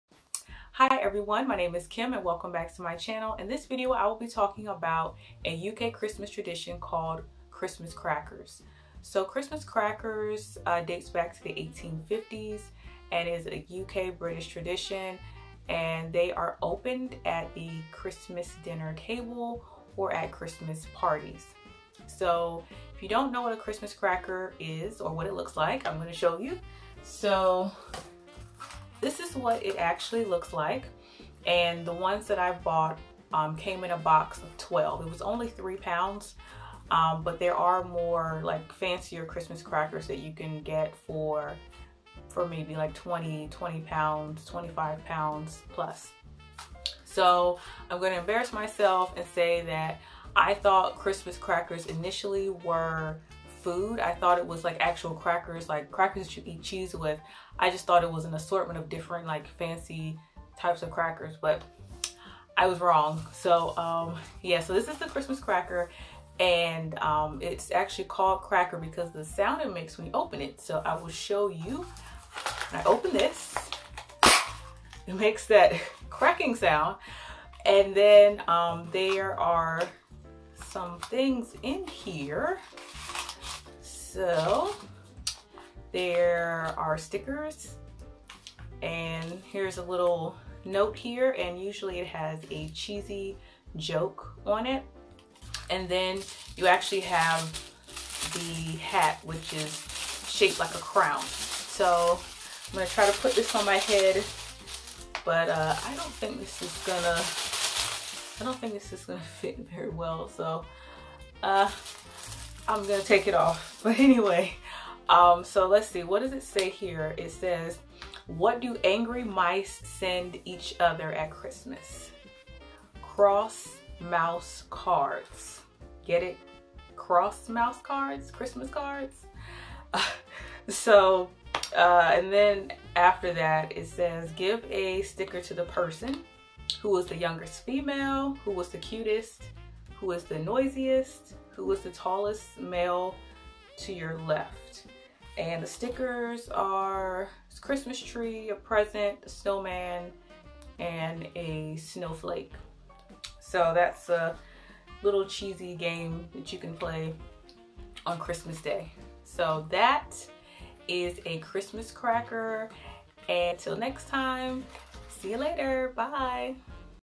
What are Christmas Crackers?| UK Christmas Tradition | American in the UK
I would see Christmas Crackers in the stores during the Christmas season but I thought they were some fancy wrapped crackers that you eat with cheese.  I finally decided to pick up a box after merely glancing at them.

Check out this video:
My First Experience using the NHS | American in England

 notquitebritish18